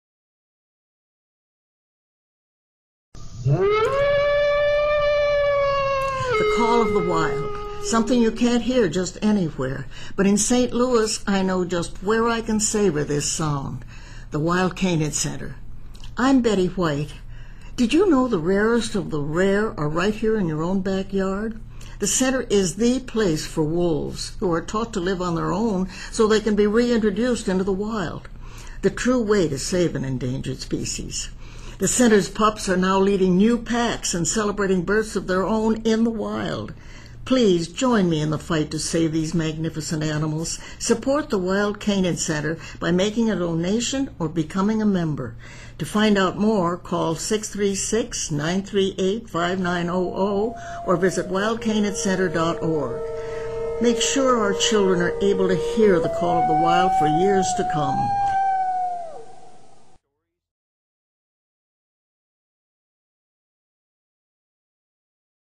Betty White supports the Endangered Wolf Center
When Betty White and Jack Hanna were guests at the Endangered Wolf Center's 2009 Gala, Jack looked at Betty as she cooed to an Albino Python and said "Is there any animal that you don't love?"  Betty didn't have to answer because the answer is clear.  Betty White isn't just a television and movie star; she's also a beacon of light and hope for animals everywhere.   Betty has been a friend and supporter of the Endangered Wolf Center for years.  As a matter of fact, Betty is actually the founder of the center's exclusive "Puppy Naming Club".  She has named several puppies and even named one very handsome male wolf 'Perkins' after the centers founder (naturalist and star of Mutual of Omaha's Wild Kingdom) Marlin Perkins. Want to find out more?